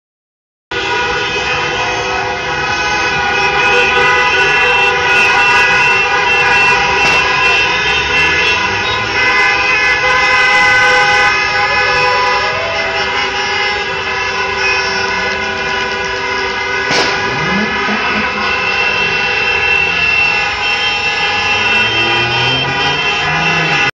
After Brazil worldcup final, in Dortmund, Germany ( Deutschland )
2014 / 07 / 14
Germany got the worldcup

After brazil worldcup final
on a street

in Dortmund, Germany